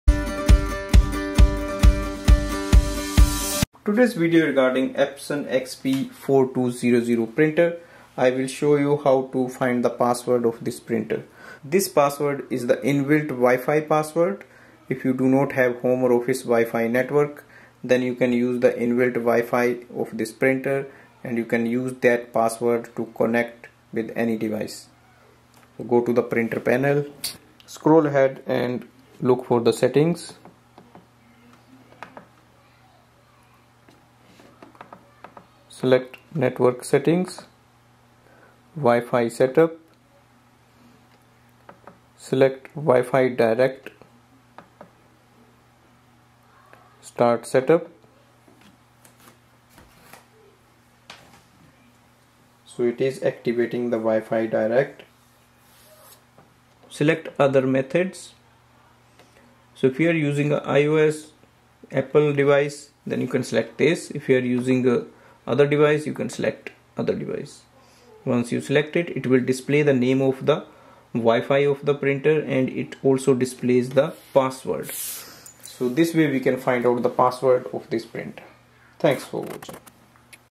Epson XP 4200 Printer WiFi Password.
This video shows how to find the password of the Epson Expression Home  XP- Printer? This is the password of the WiFi Direct settings.